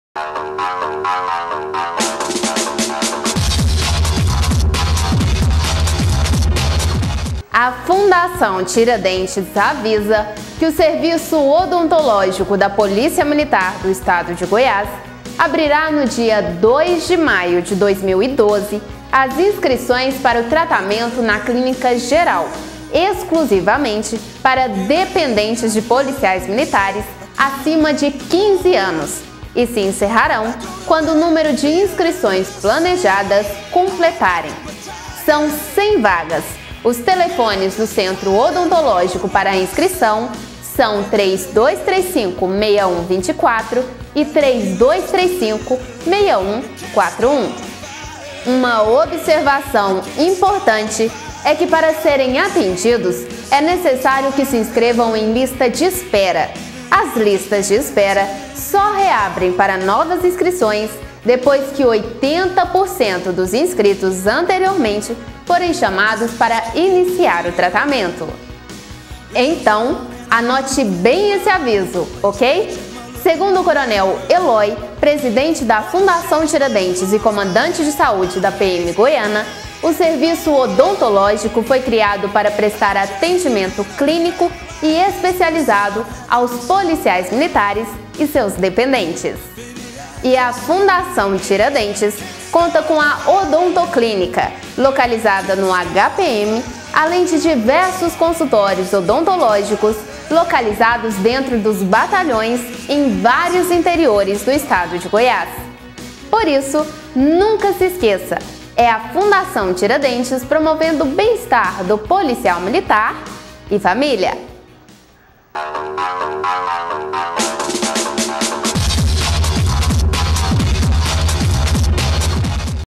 Atenção, o Serviço Odontológico oferece 100 vagas para dependentes
A Fundação Tiradentes avisa que o Serviço Odontológico da Polícia Militar do Estado de Goiás abrirá no dia 02 de maio de 2012 as inscrições  para tratamento na Clínica Geral, exclusivamente para dependentes de policiais militares acima de 15 anos, e se encerrarão quando o número de inscrições planejadas completarem. São100 vagas, Os telefones do Centro Odontológico para a inscrição são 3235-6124 e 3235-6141. Uma observação importante é que para serem atendidos é necessário que se inscrevam em lista de espera. As listas de espera só reabrem para novas inscrições, depois que  80%  (oitenta por cento) dos inscritos anteriormente forem chamados para iniciar o tratamento. Então anote bem esse aviso OK. Segundo o coronel Elói, presidente da Fundação Tiradentes e comandante de Saúde da PM Goiana, o serviço Odontológico foi criado para prestar atendimento clínico e especializado aos Policiais Militares e seus dependentes. E a Fundação Tiradentes conta com a Odontoclínica, localizada no HPM - além de diversos consultórios odontológicos localizados dentro dos batalhões em vários interiores do Estado de Goiás. Por isso, nunca se esqueça, é a Fundação Tiradentes promovendo o bem estar do policial militar e família.